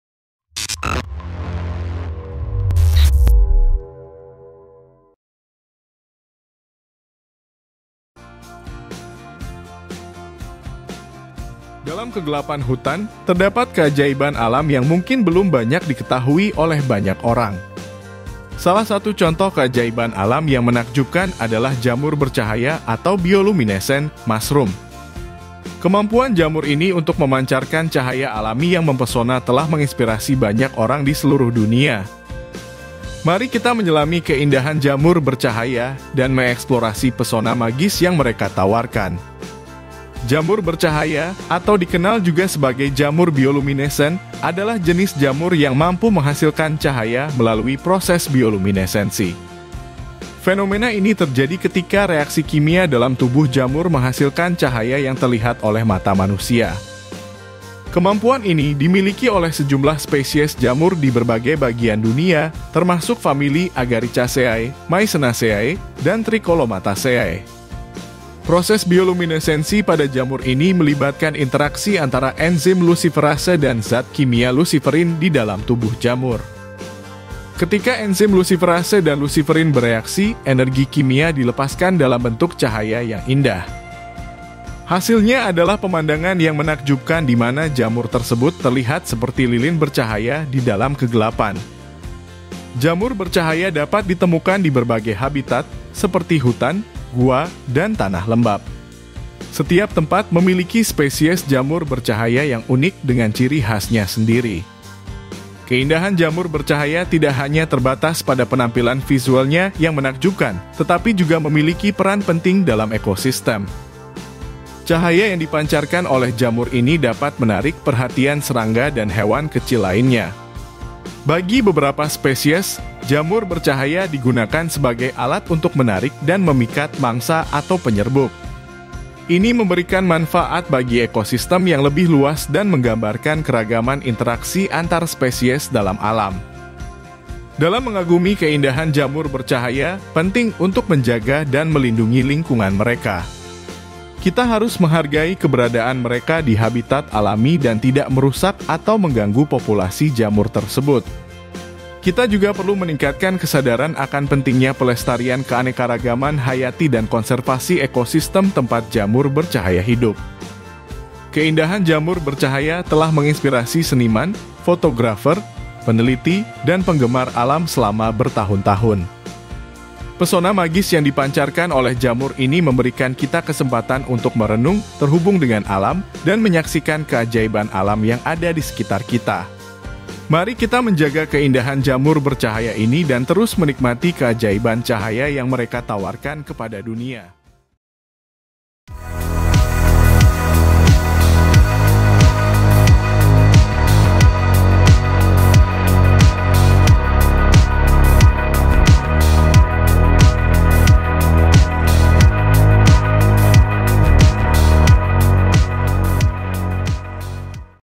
MENAKJUBKAN, Jamur Ini Bisa Bercahaya Dalam Gelap | Bioluminescent Mushrooms
Terima Kasih Telah Menyaksikan

Dalam keindahan alam yang luar biasa, terdapat fenomena yang begitu menakjubkan dan mempesona, yaitu Bioluminescent Mushroom atau jamur bercahaya. Keindahan jamur ini terletak pada kemampuannya untuk memancarkan cahaya sendiri, menciptakan pemandangan yang begitu memukau di dalam kegelapan malam. Mari kita telusuri keajaiban Bioluminescent Mushroom yang memikat hati dan mempesona mata kita.
Bioluminescent Mushroom, juga dikenal sebagai jamur bioluminescent, adalah jenis jamur yang mampu menghasilkan cahaya alami melalui proses bioluminesensi. Keunikan ini dimiliki oleh sejumlah spesies jamur di seluruh dunia, termasuk dalam keluarga Agaricaceae, Mycenaceae, dan Tricholomataceae. Walaupun beragam dalam bentuk dan ukuran, mereka memiliki satu hal yang sama: kemampuan mereka untuk memancarkan cahaya yang menakjubkan.
Proses bioluminesensi pada jamur ini melibatkan interaksi antara enzim luciferase dan zat kimia luciferin di dalam tubuh jamur. Ketika enzim luciferase bereaksi dengan luciferin, energi kimia dilepaskan dalam bentuk cahaya. Dalam kegelapan malam, jamur ini terlihat seperti bintang-bintang yang jatuh dari langit, menciptakan suasana ajaib di sekitarnya.
Salah satu lokasi terkenal untuk menyaksikan keindahan Bioluminescent Mushroom adalah Hutan Nasional Daisetsuzan di Jepang. Pada musim panas dan musim gugur, ribuan jamur bercahaya bermekaran di bawah pepohonan, menciptakan pemandangan yang spektakuler. Cahaya yang dipancarkan oleh jamur ini membentuk panorama yang magis, seolah kita memasuki dunia fantasi.
Namun, keindahan Bioluminescent Mushroom tidak hanya terbatas pada Jepang. Jamur ini juga dapat ditemukan di berbagai belahan dunia, seperti Amerika Serikat, Australia, Amerika Selatan, dan Eropa. Setiap tempat memiliki spesies jamur bercahaya yang unik dengan karakteristik dan warna yang berbeda.
Tidak hanya mempesona secara visual, Bioluminescent Mushroom juga memiliki peran penting dalam ekosistem. Cahaya yang dipancarkannya mampu menarik perhatian serangga dan hewan kecil lainnya, yang pada gilirannya menjadi sumber makanan bagi jamur tersebut. Selain itu, beberapa spesies jamur bercahaya mengandung senyawa kimia potensial yang memiliki sifat antibakteri dan antitumor, menjadikannya menarik dalam penelitian biomedis.
Namun, dalam mengagumi keindahan Bioluminescent Mushroom, kita juga harus menjaga dan melindungi lingkungan mereka. Kita harus menghindari merusak jamur atau ekosistem di mana mereka tumbuh. Sebagai pengunjung, kita dapat berperan sebagai penjaga alam dan meningkatkan kesadaran akan keindahan serta pentingnya keanekaragaman hayati.
Bioluminescent Mushroom adalah contoh nyata keajaiban alam yang ada di dunia ini. Dengan kemampuan mereka untuk memancarkan cahaya yang indah, jamur ini tidak hanya memikat mata, tetapi juga membangkitkan rasa ingin tahu dan kekaguman manusia terhadap keanekaragaman alam. Mari kita terus menghargai dan menjaga keindahan ini agar generasi mendatang juga dapat menikmati pesona cahaya yang diberikan oleh Bioluminescent Mushroom.